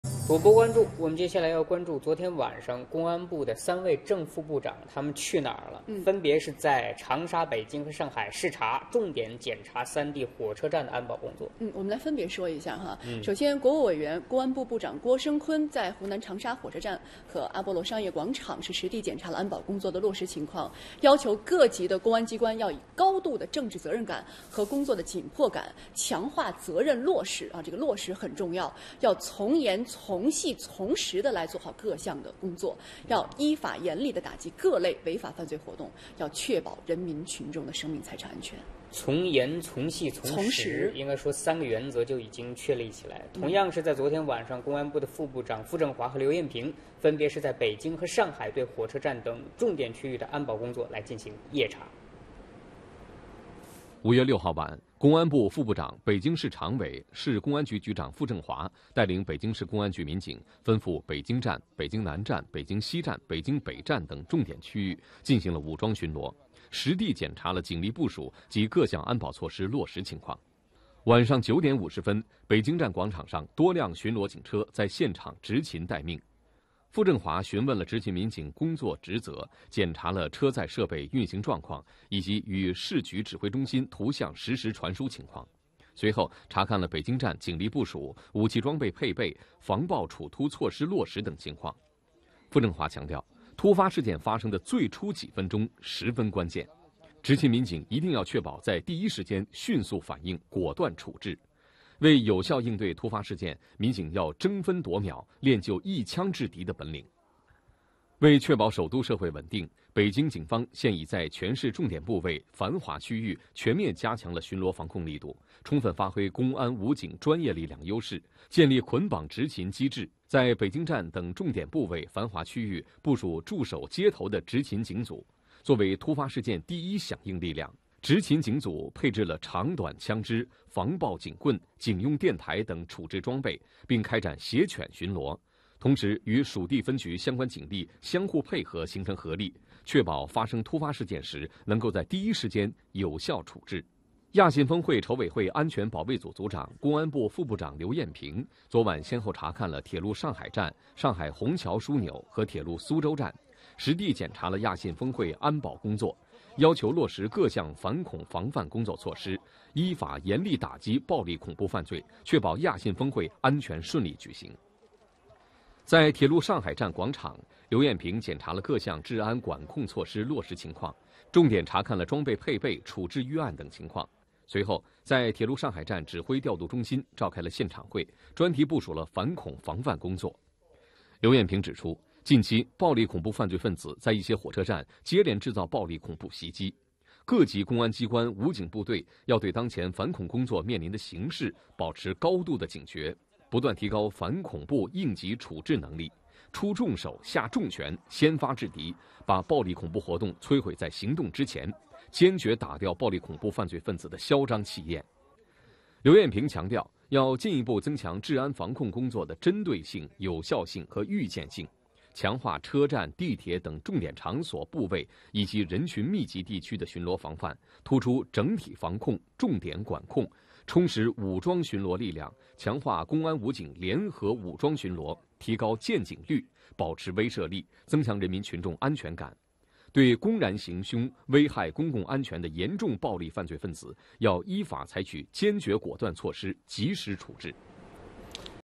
公安部正副部長三人分別巡視火車站 要求一槍制敵
廣州火車站廣場發生斬人事件後，公安部長郭聲琨以及兩名副部長傅政華、劉彥平分別在長沙、北京、上海火車站巡視保安工作，其中傅政華更配備短槍進行武裝巡邏，對現場民警說：「突發事件發生的最初幾分鐘十分關鍵，執勤民警一定要確保在第一時間迅速反應，果斷處置，掌握主動」，「槍法一定要準，要練就一槍制敵的本領」。